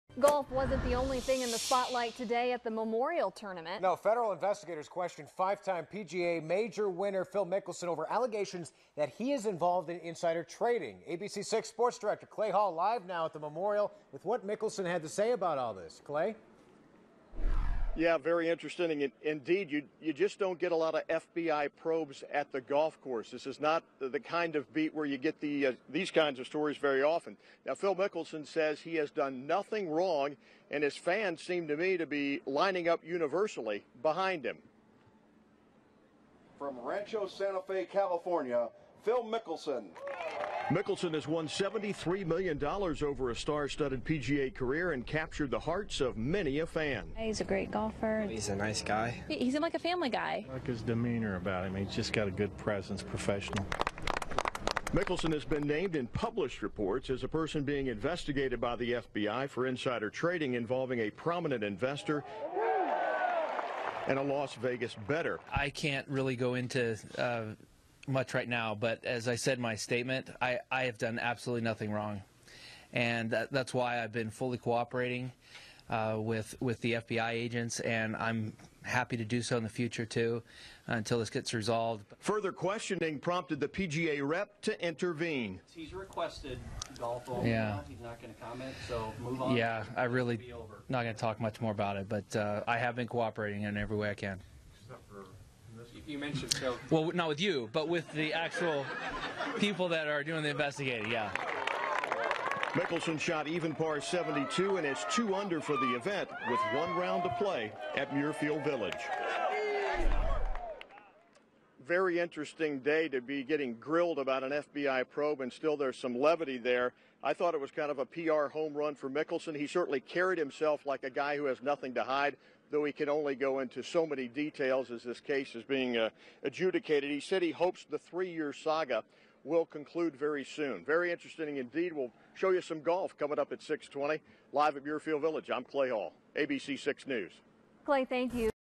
PGA's Mickelson Cooperating in FBI Probe
DUBLIN, OH --PGA great Phil Mickelson says he's cooperating with a federal
investigation into possible insider trading involving billionaire
investor Carl Icahn and Las Vegas gambler William "Billy" Walters.

In
 a statement early Saturday and after playing the third round of The Memorial tournament at Muirfield Village GC, Mickelson said he was fully cooperating with the Federal Bureau
of Investigation and the Securities and Exchange Commission.

ABC-6/FOX-28 Sports Director Clay Hall reports.